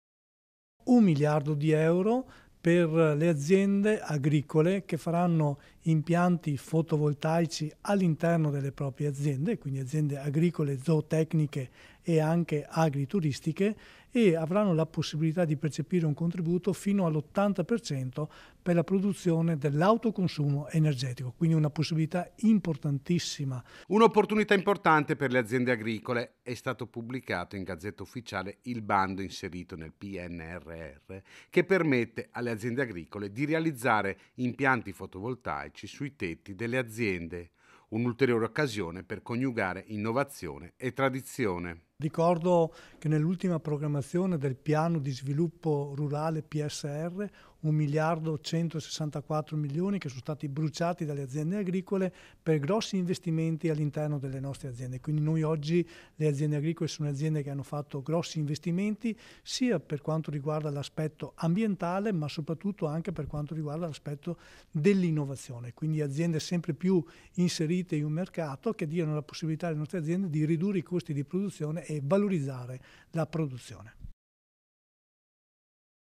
FOTOVOLTAICO IN AGRICOLTURA: UN BANDO PER LE AZIENDE | 09/07/2023
A3 NEWS Treviso 09/07/2023 - PADOVA - Da settembre sarà possibile accedere al bando che finanzia l'installazione di pannelli fotovoltaici sui tetti delle aziende agricole. Un'occasione per le realtà agricole di diventare energeticamente autosufficiente ottenendo un rimborso a fondo perduto dell'80% della spesa.  || Un'opportunità importante per le aziende agricole. E' stato pubblicato in gazzetta ufficiale il bando, inserito nel PNRR che permette alle aziende agricole di realizzare impianti fotovoltaici sui tetti delle aziende. Un'ulteriore occasione per coniugare innovazione e tradizione. - Intervistati MAURIZIO ANTONINI (Direttore CIA Padova) (Servizio di Francesco Rataj) - Segui Antenna Tre anche sul digitale terrestre! Visita il sito